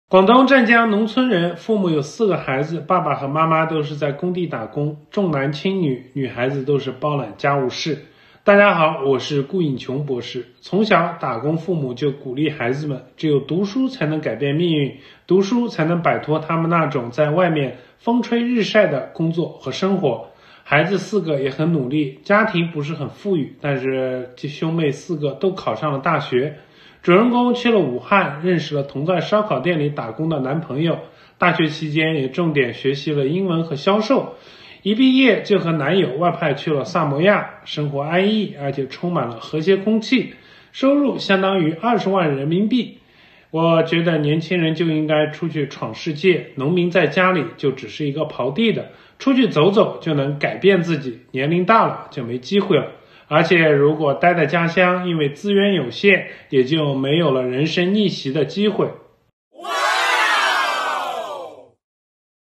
99年广东农村美女和男友被外派萨摩亚年入20万人生逆袭
99年广东农村美女和男友被外派萨摩亚年入20万人生逆袭